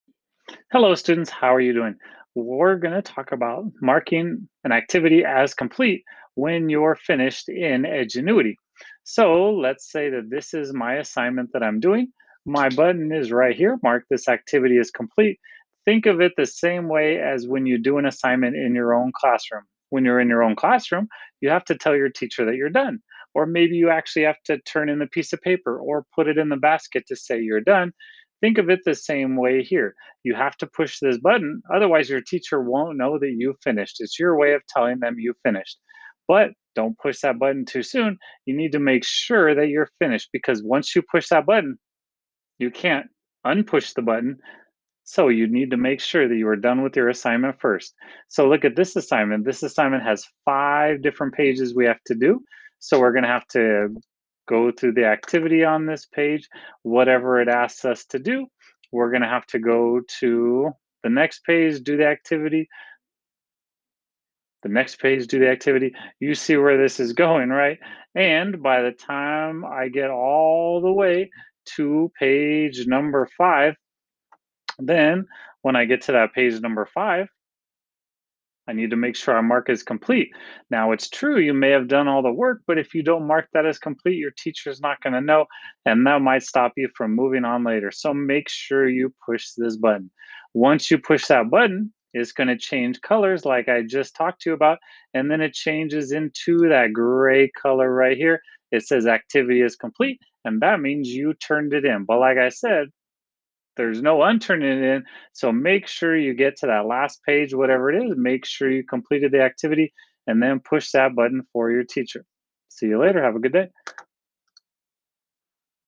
Student Tutorial - Edgenuity - Mark as Complete
This tutorial shows students how to mark an assignment as complete in Edgenuity.